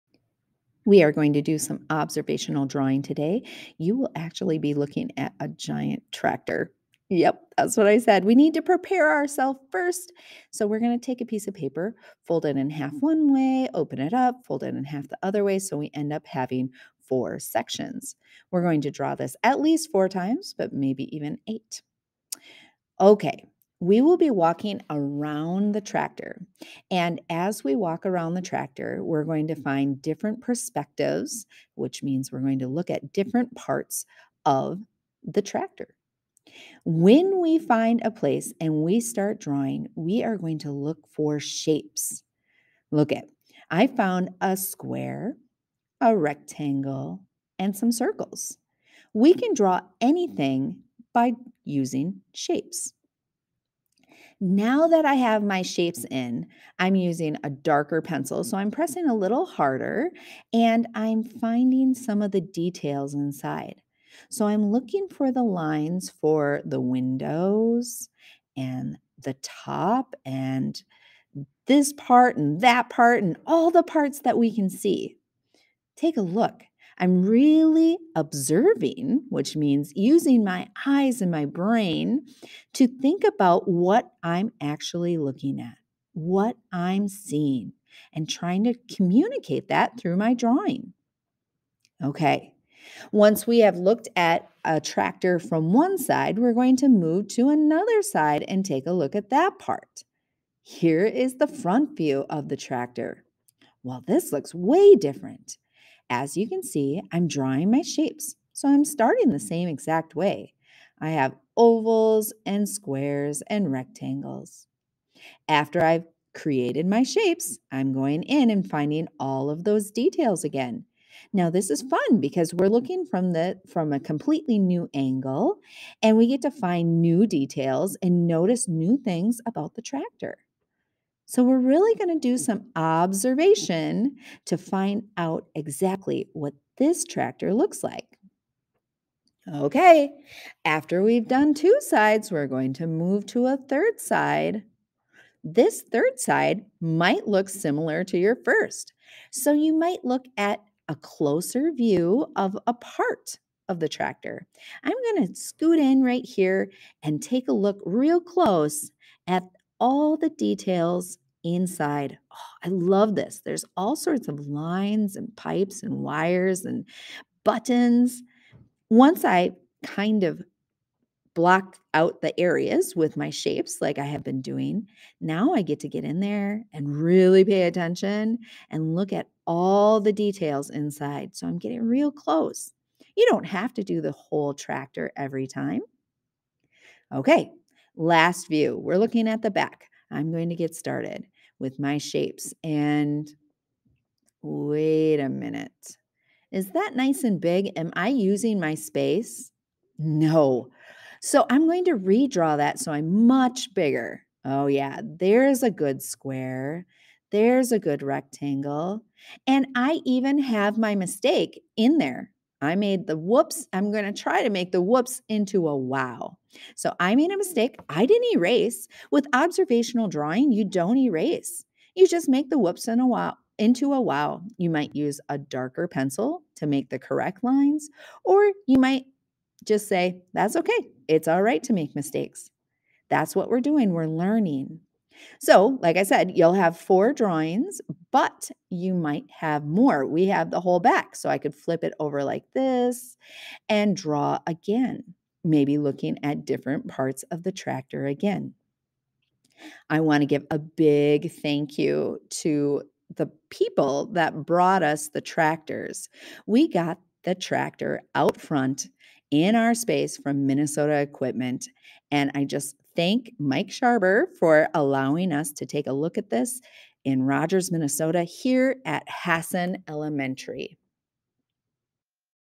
Tractor Observational Drawing
We are so lucky to end our Farm Unit by drawing a tractor. Minnesota Equipment is going to park a tractor outside of the art room and we are going to spend our Art time outside, drawing and enjoying the Spring weather. Really excited for this event.